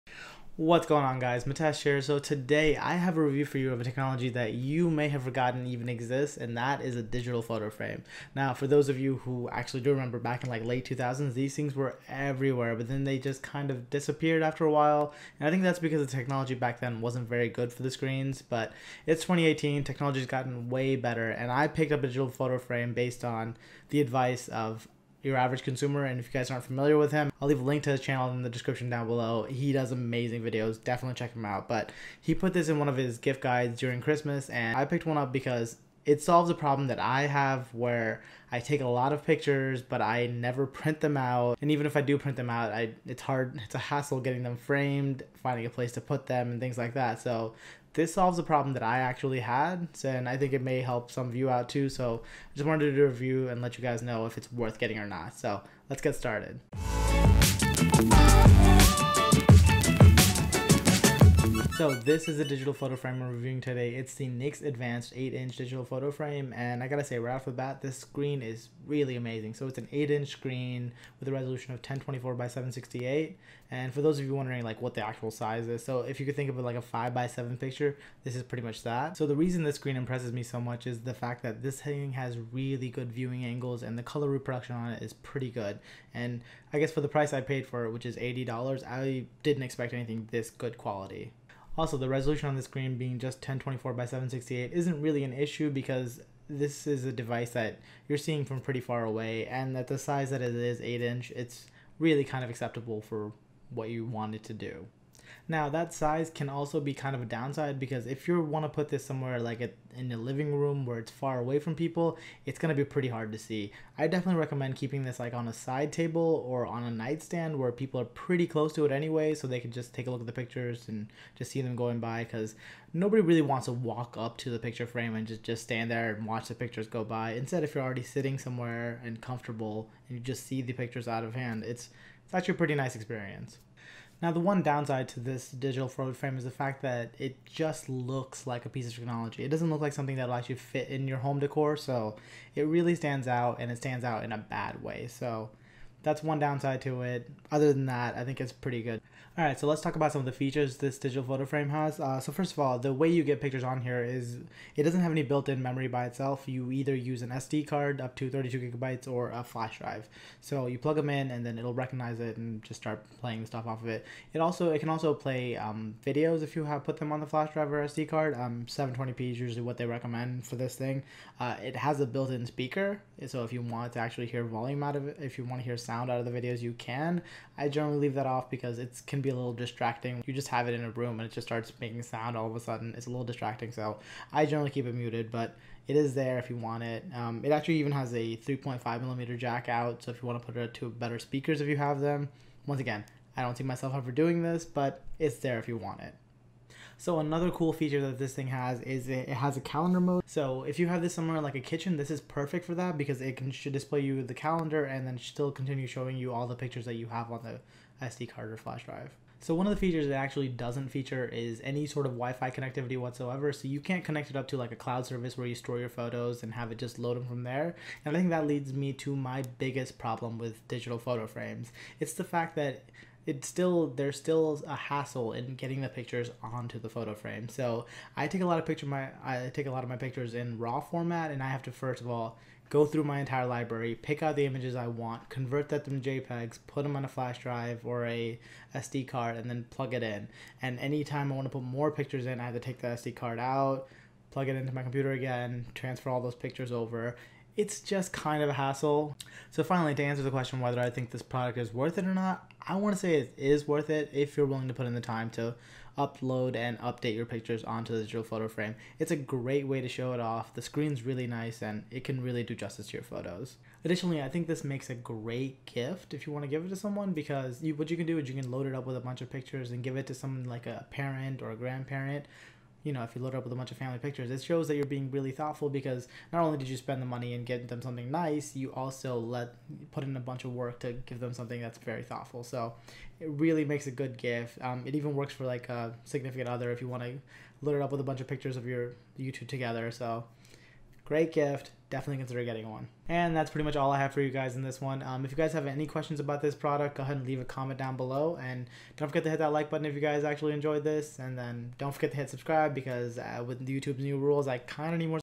Nix Advance Digital Photoframe Review
Review of the Nix Advance 8 inch digital photo frame with motion sensor. Is it worth buying, has the technology improved from the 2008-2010 days when they were popular.

Nix Advance - 8 inch hi-res digital photo frame with motion sensor (x08e)